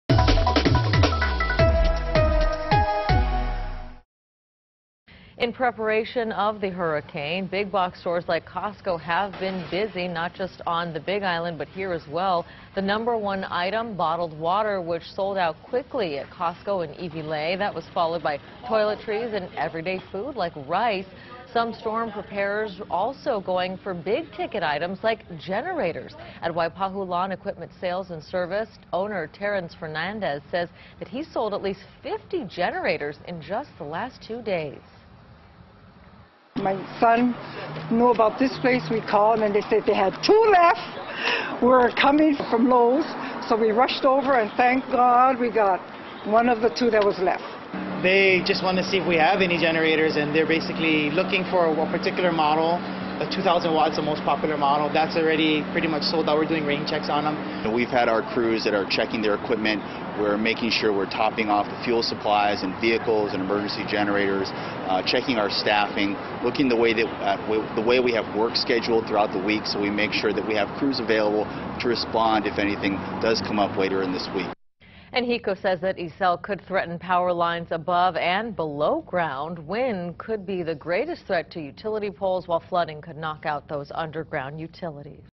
Oahu residents wait in lines for storm supplies
Long lines at stores like Costco were common on Tuesday as Oahu residents prepare for the storm.